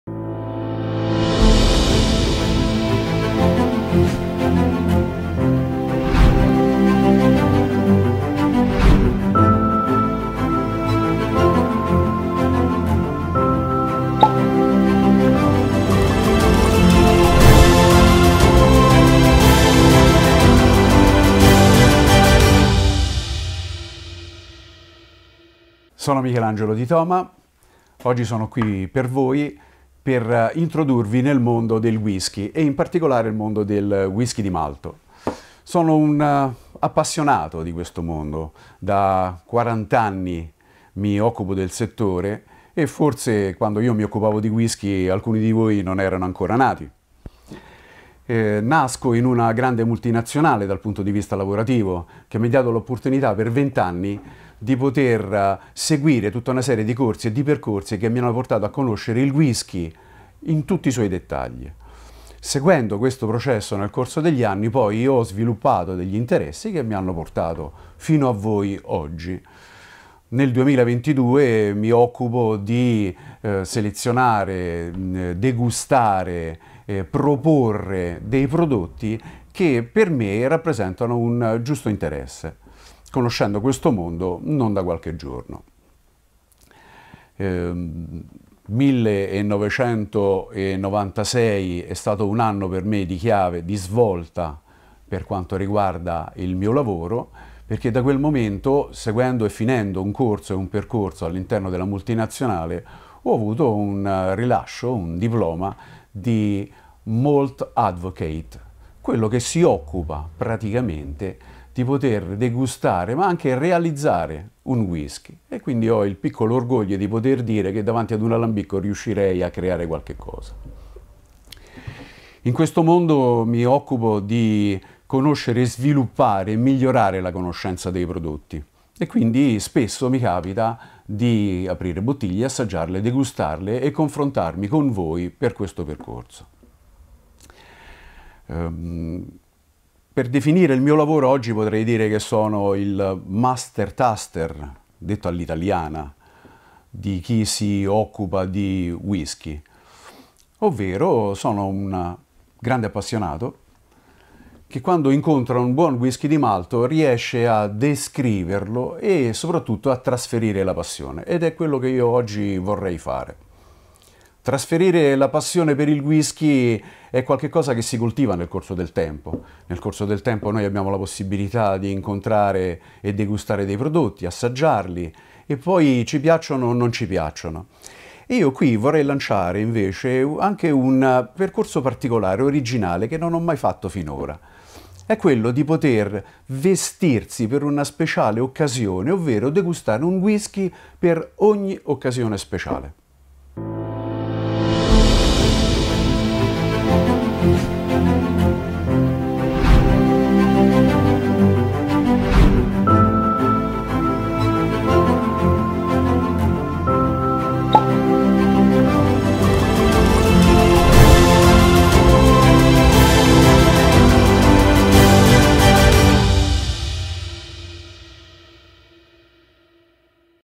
Intervista Michelangelo Di Toma
Aquavitae il miglior modo di bere Whisky
Splendido incontro con Michelangelo Di Toma che ci descrive i prodotti di Wilson&Morgan e Ian MacLeod Distillers
Wilson&Morgan
Dal 1992 inizia il percorso della famiglia Rossi con Wilson&Morgan, marchio italiano che si pone l'obiettivo di ricercare e la selezionare accuratamente solo whisky unici e ricchi di personalità, i migliori Single Malt Scotch Whisky.
Abili imbottigliatori capaci di scegliere fra le differenti distillerie, selezionando il giusto Single Malt nel momento più inteso della sua maturazione, affinando i whisky selezionati in barili che hanno contenuto altri spiriti, come il Rum, o importanti vini (Marsala, Porto, etc.) per arricchirne il profilo aromatico e il gusto.
Una collezione creata da intenditori per veri appassionati: nei whisky Wilson&Morgan si ritrova l’intensità e il carattere genuino delle tipiche zone di produzione scozzesi, dalla torba dell’isola di Islay, alla morbidezza dei più gentili e dolci malti dello Speyside.

Ian MacLeod Distillers
La Ian Macleod Distillers nasce nel 1933, a Broxburn, da Leonard J. Russel, amante del Whisky, che inizialmente selezionava botti da diverse aziende scegliendo solo quello che riteneva più idoneo. Nel corso degli anni si è affermata come imbottigliatore indipendente acquisendo distillerie come Glengoyne, Tamdhu, e marchi come Isle of Skye. Nei suoi oltre 80 anni di storia ha anche sviluppato una serie di etichette, come lo Smokehead, che è un whisky torbato di Islay.
Oggi l'azienda è tra le più grandi dieci al mondo come produttore di whisky, infatti produce attualmente oltre 15 milioni di bottiglie ogni anno.
Aquista ora:
Produttore Marco Casavecchia Aquavitae
Regia albertosikera
Drink Responsibly
DRINKiQ.com
Visita:
Tutti i diritti riservati Copyright
Visita:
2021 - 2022 Numero di licenza 26...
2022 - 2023 Numero di licenza 55...